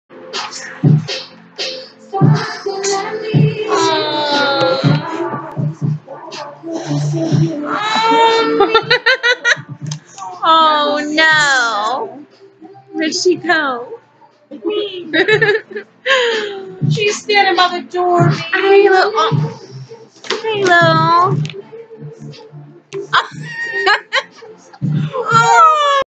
Balloon Fears!
Great Dane is afraid of balloons